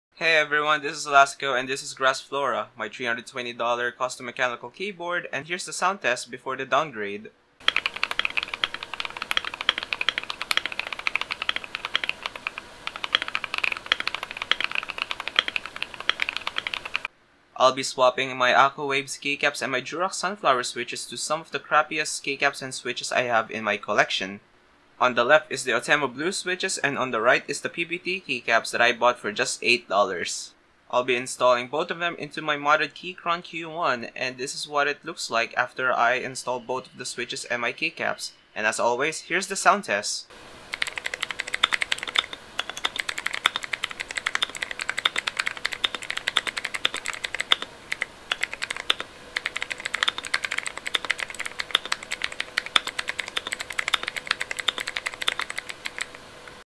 MODDED KEYCHRON Q1 with the CRAPPIEST switches and keycaps
Keyboard setup: BEFORE
Keyboard: Keychron Q1 (With Polycarbonate plate)
Switches: Durock T1 POM Sunflowers (Lubed with "205G0" and Filmed with KBDFan's PC film)
Keycaps: AKKO Waves PBT ASA Profile Keycaps

Keyboard setup: AFTER
Switches: Outemu blue stock
Keycaps: $8 Cheap PBT keycaps

Mods:
-Rubber and double-sided tape in the middle of top and bottom housing
-Two layers of PE foam (1mm and 0.5mm thick)
-Tape mod (two layers of masking tape and one layer of electrical tape)
-Added KBDFans's switch and stab foam pads
-Relubbed the stabs.